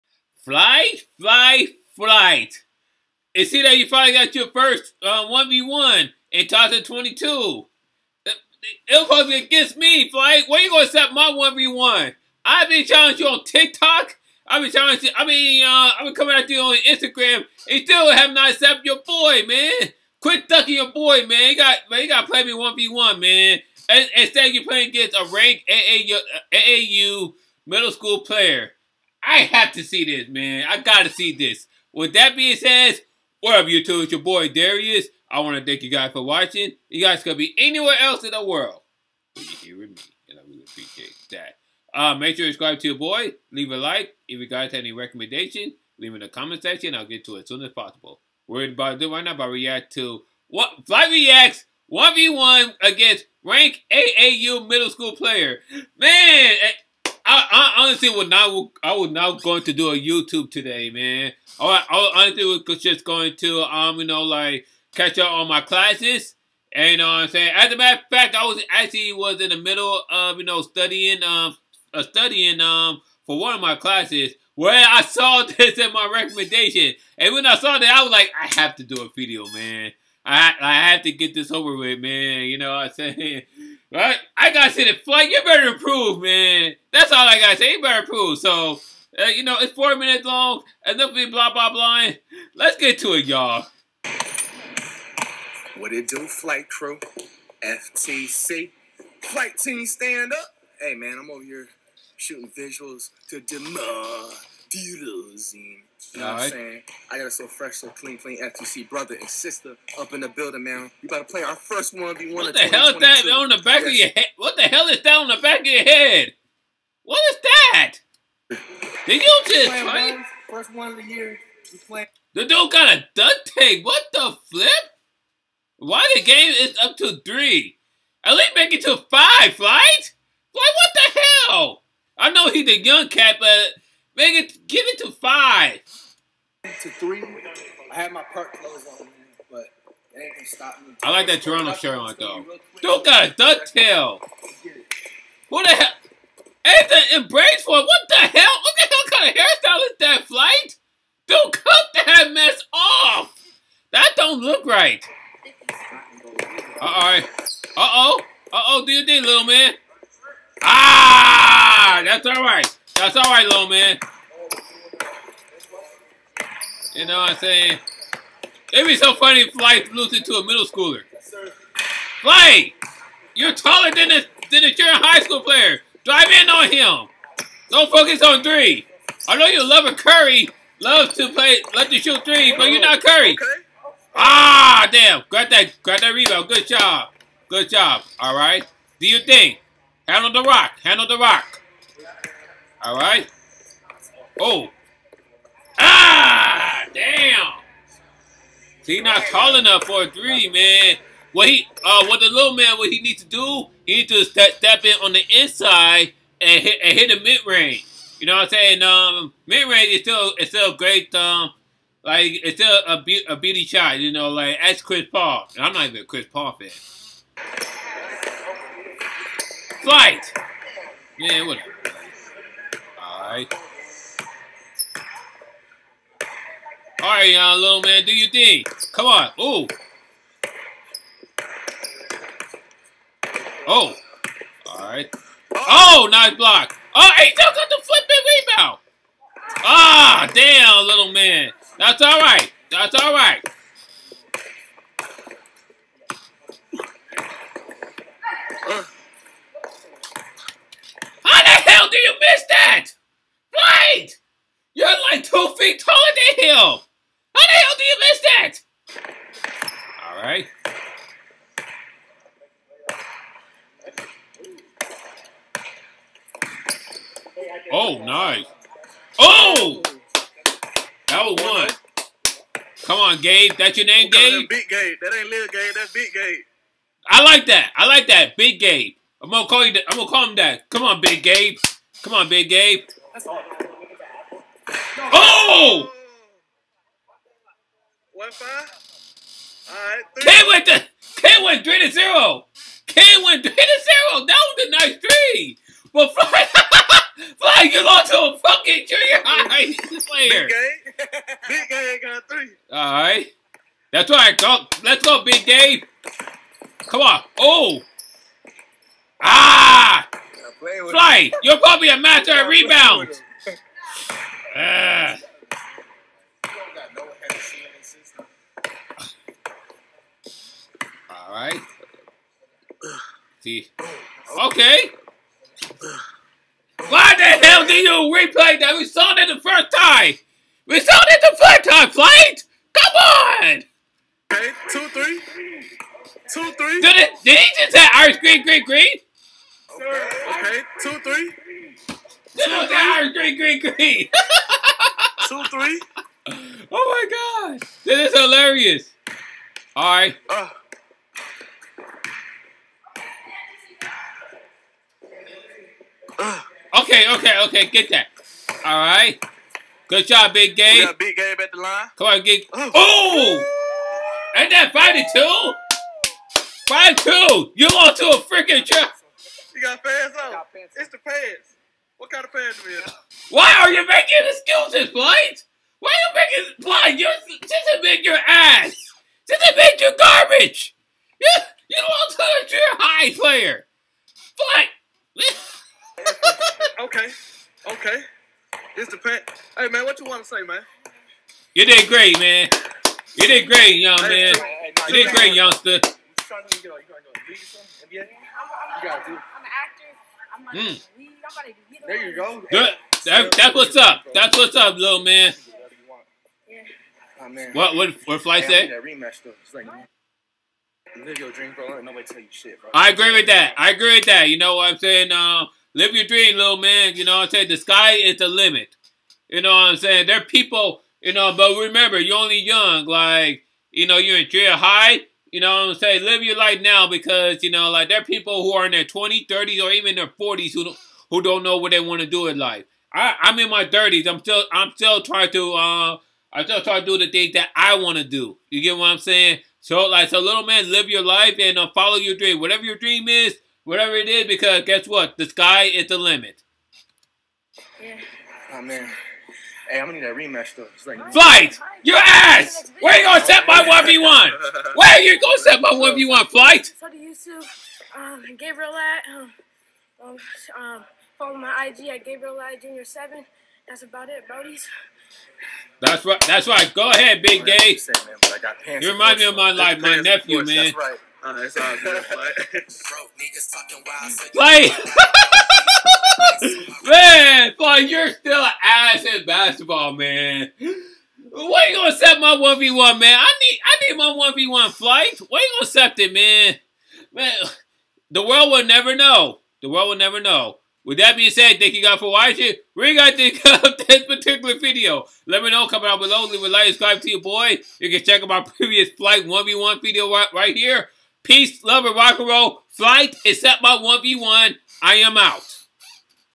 Flightreacts Plays 1v1 Against a Middle Schooler and DJH88 Could Not Stop Laughing
Flightreacts is back with a 1v1, this time with a Middle Schooler and as you guess it, he's still trash. With that being said, that was a great words to the young kid. Major respect to Flight.

Snapshat - dariushenry88